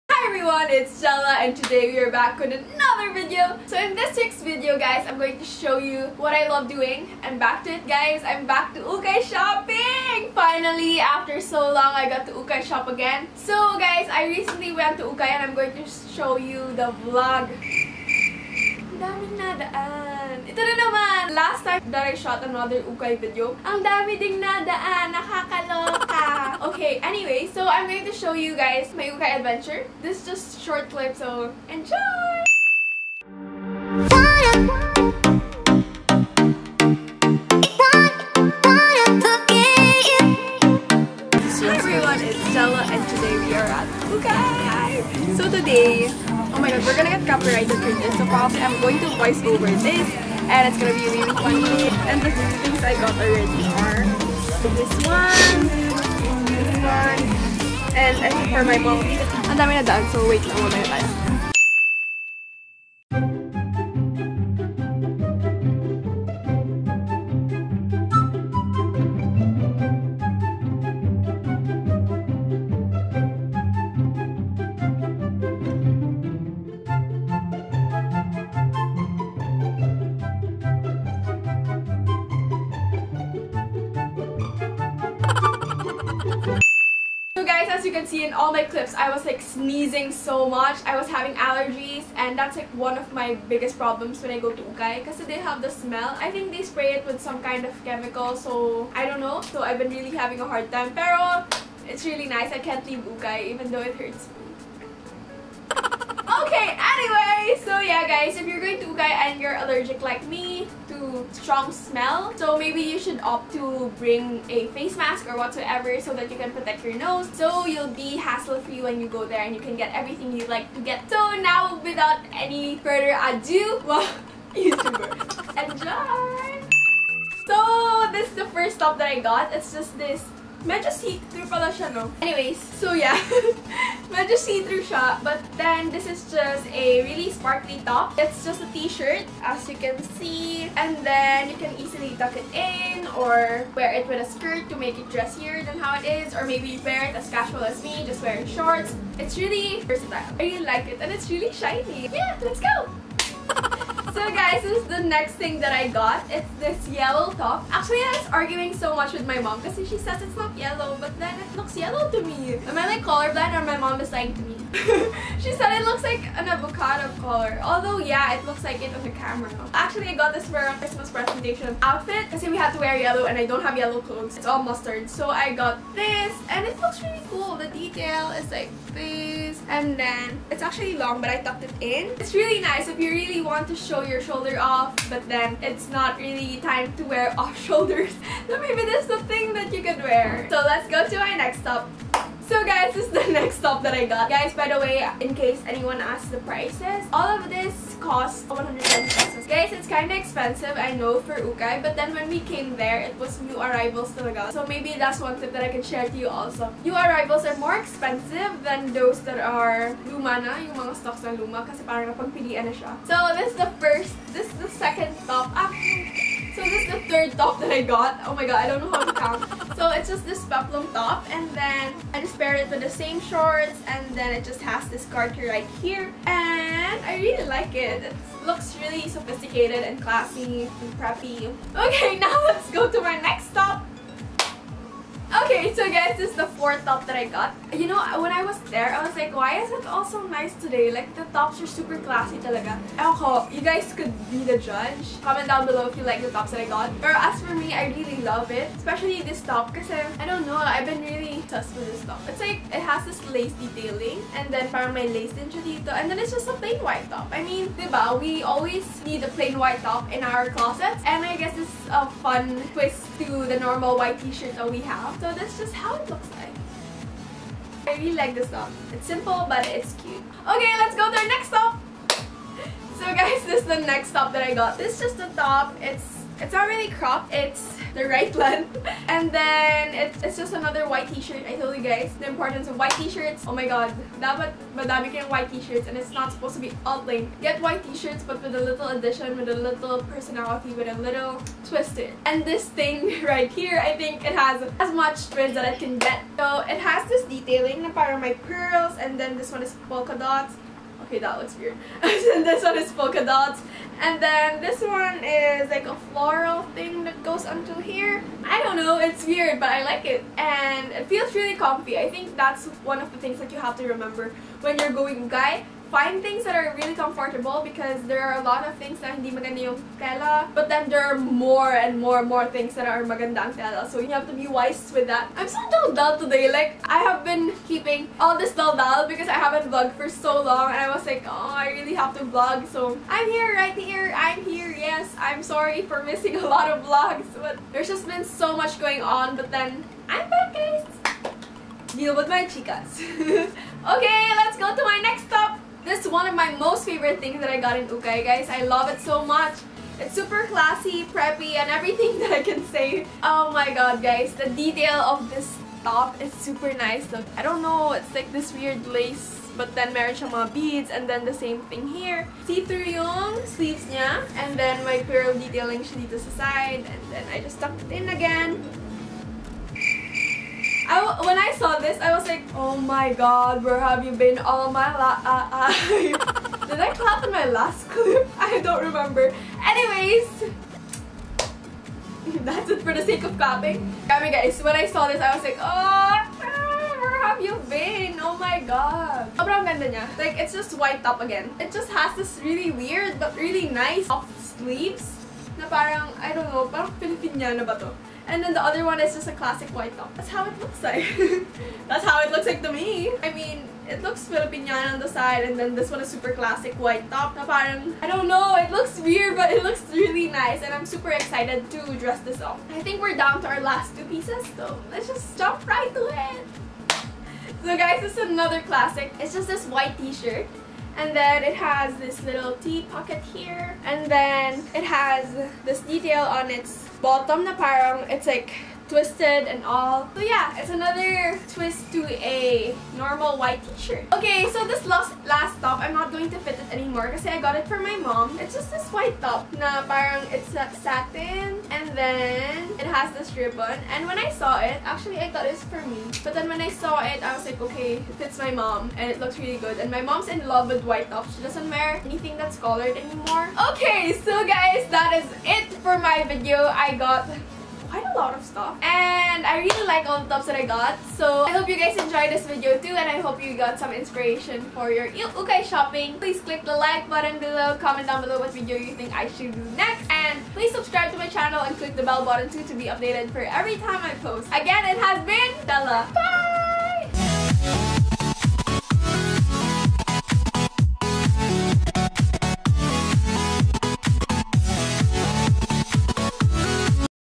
Ukay-Ukay Vlog + Try-On Haul 2019!!!｜GELA MACALALAD
hi everyone! it's gela! i missed you guys. btw, merry christmass!! here's my early present to you all, an ukay-ukay vlog+ try on haul. wahhh feels so good to be back ukay-ing!! hope you enjoy the vid ❤️

love,
gela (mejj vlogger)